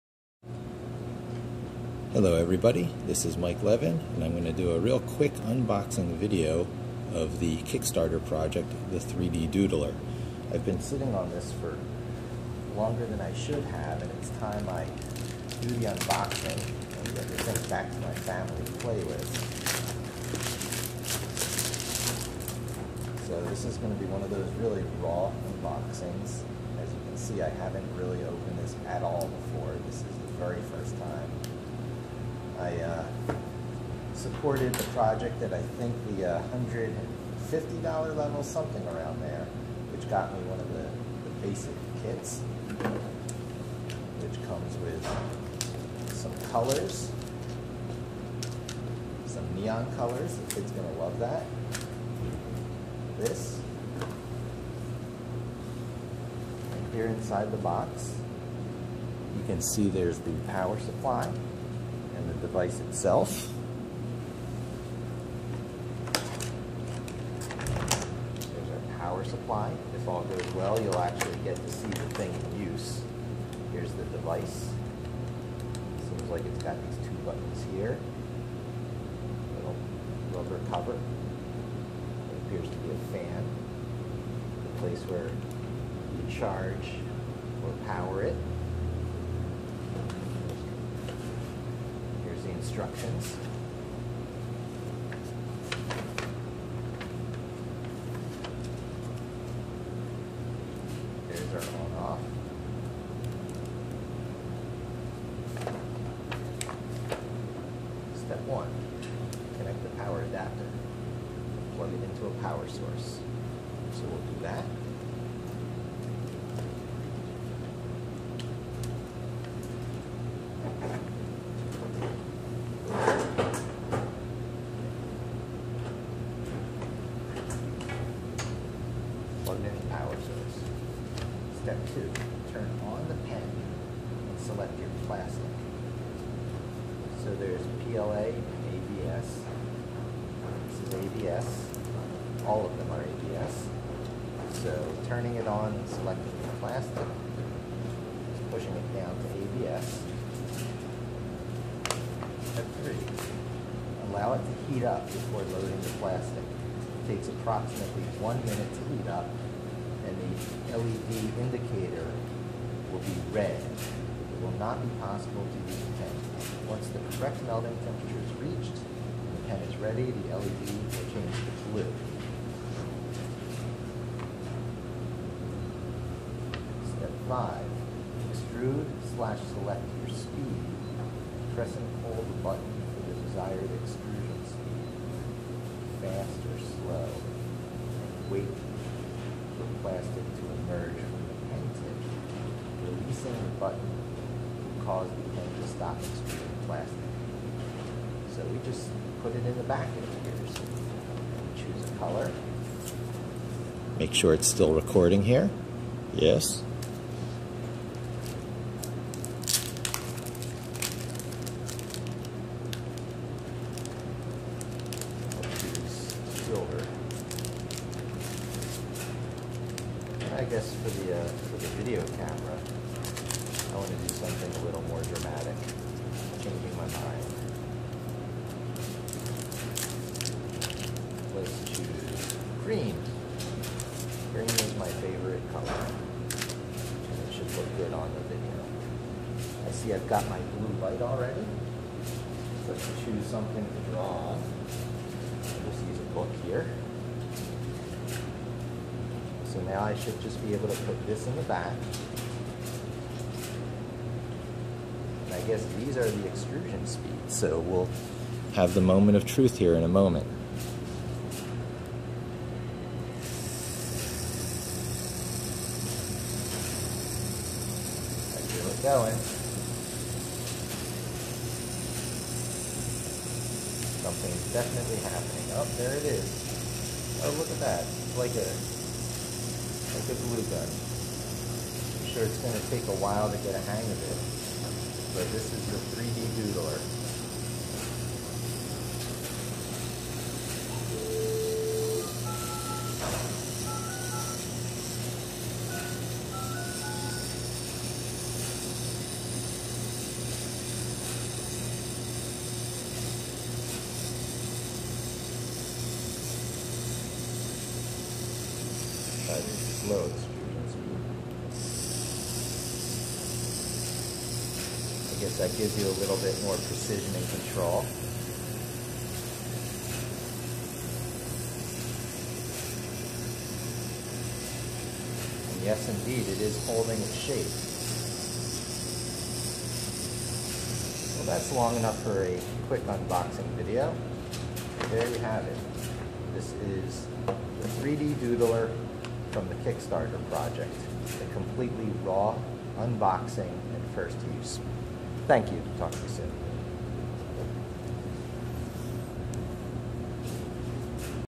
Kickstarter 3Doodler Unboxing
— Completely raw unboxing of the Kickstarter-funded 3Doodler